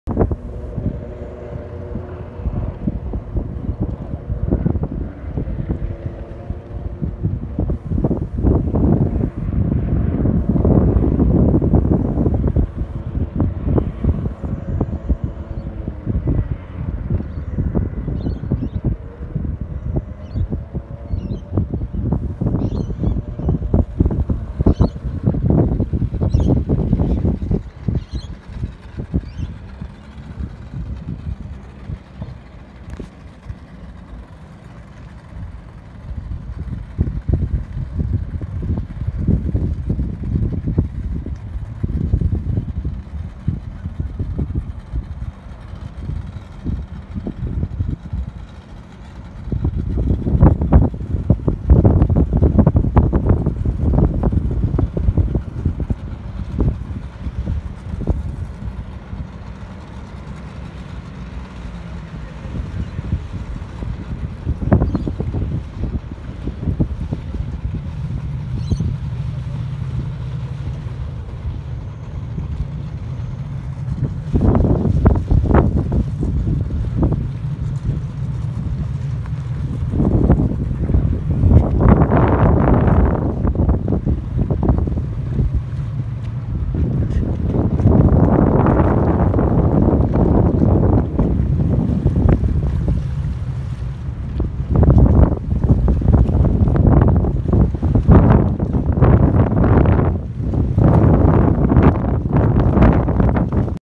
Lane Vale George’s River
Test run Yamaha jet boat at Lansvale